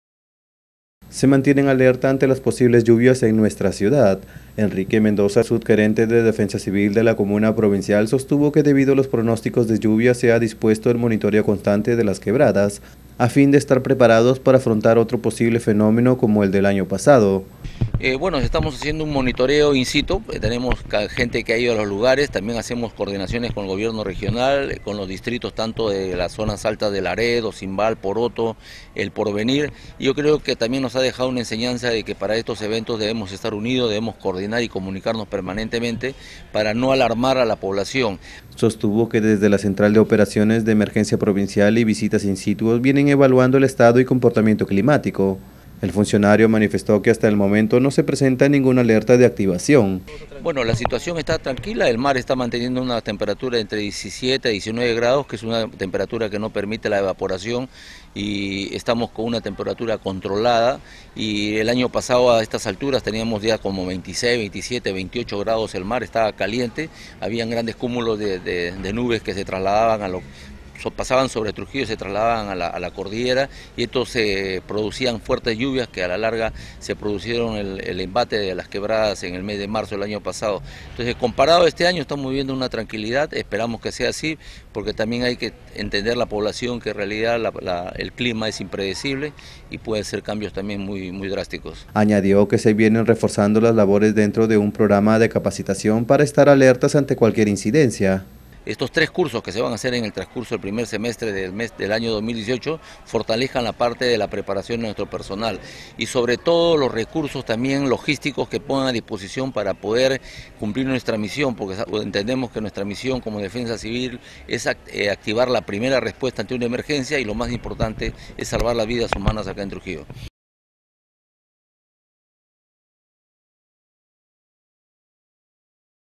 NOTA Defensa Civil realiza diariamente monitoreo de quebradas
El Subgerente de Defensa Civil, Enrique Mendoza, informó que a diario se realiza el monitoreo del comportamiento climático en las quebradas. Asimismo, recomendó a la población mantenerse alerta ante posibles lluvias que anunció SENAMHI.